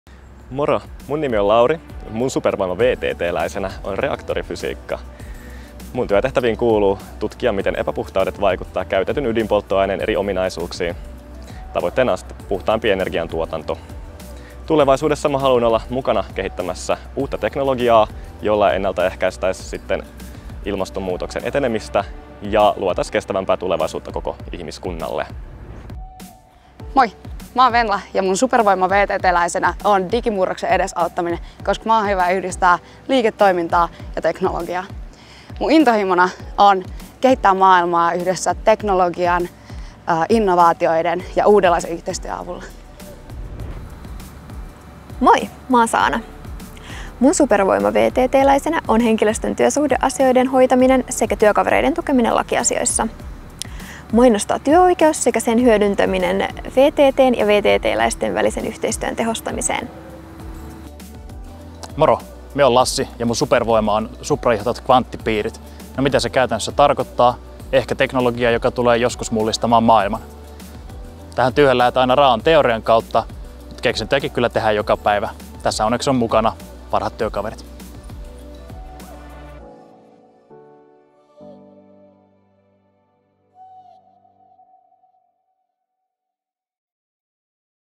Vastuullinen kesäduuni ja supervoimat
Meillä VTT:llä pääsee jo kesätöissä ja harjoittelussa hyödyntämään omia kykyjä ja taitoja. Katso videolta, mitä Lauri, Venla, Saana ja Lassi kertovat supervoimistaan VTT:läisinä!